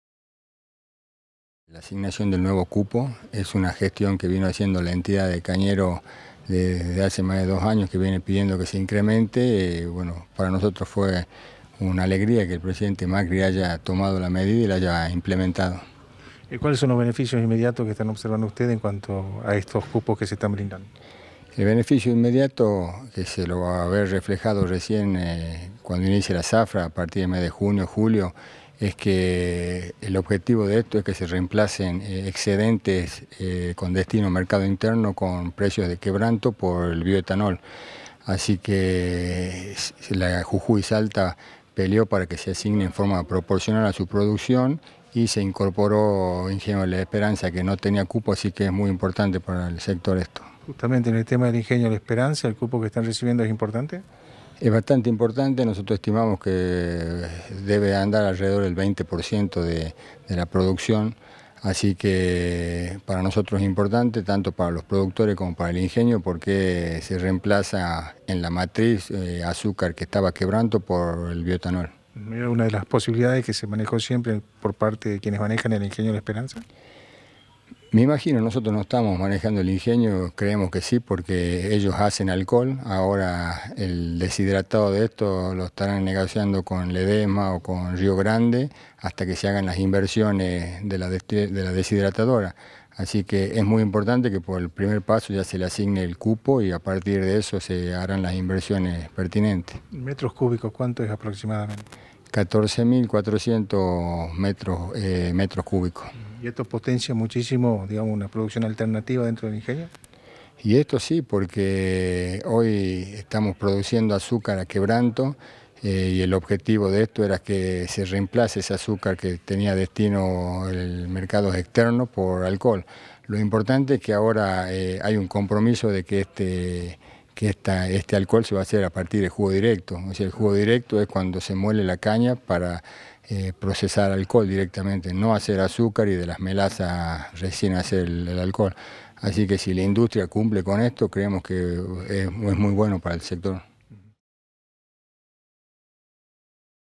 Jujuy y Salta beneficiados con las modificaciones del cupo al Bioetanol
José María Jure, representante de la Unión de Cañeros de Jujuy brindó detalles sobre este beneficio para el sector.